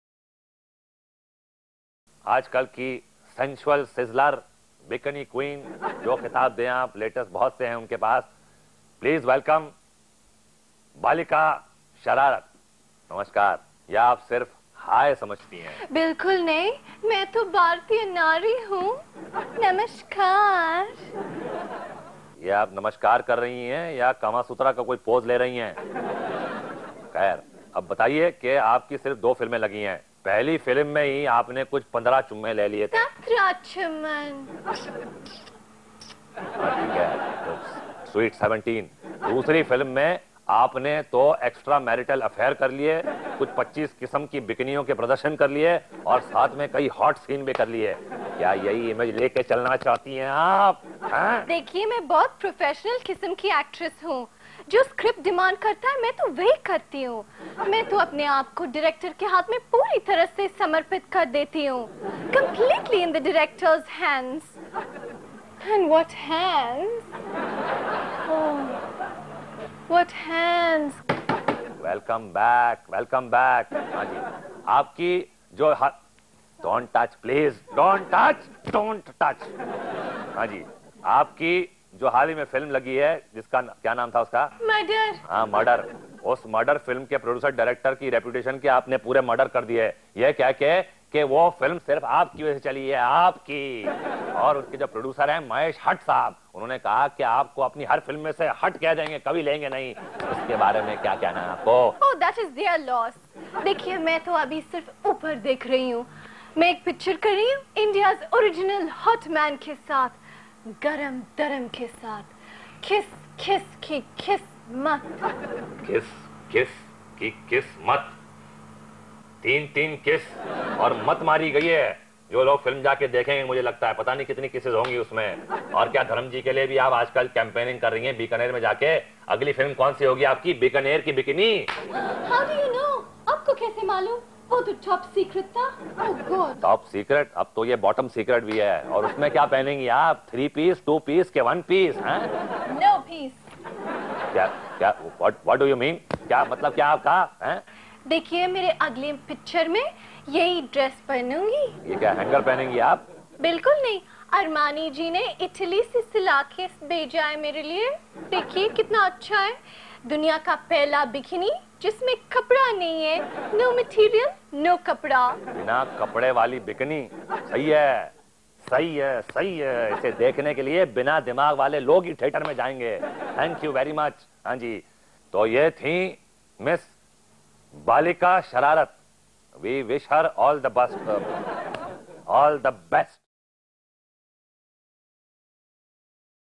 BHAIRAVI GOSWAMI VERY SEXY & GLAMOROUS
Bollywood Actress Bhairavi talks about her bikinis, on screen , kisses & more a La Ms. Sherawat.